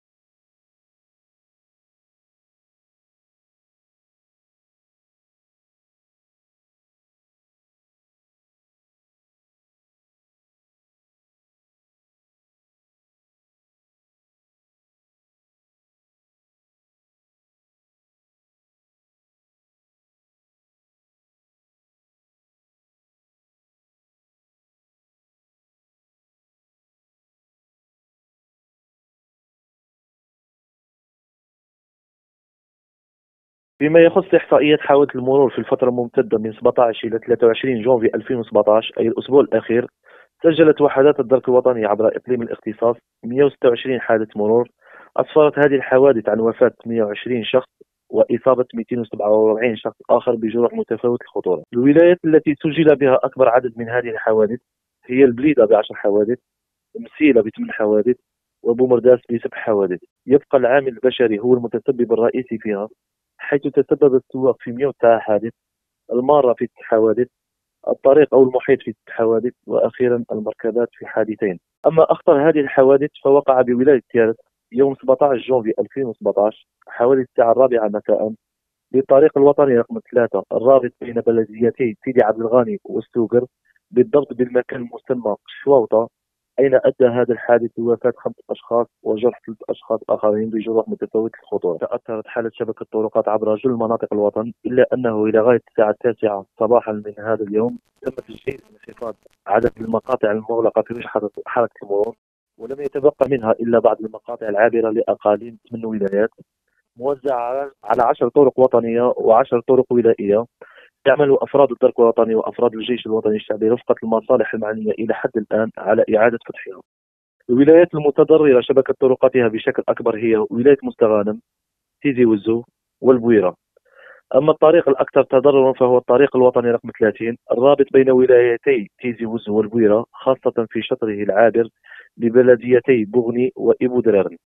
28 قتيلا و 278 جريحا في الحوادث المرور خلال هذا الأسبوع
تابعوا قناة الشروق الإخبارية على الترددات التالية :a033 H SR:5000 | HD 10992 SR:5000
أو عبر الوسائط التالية : مباشر من قسنطينة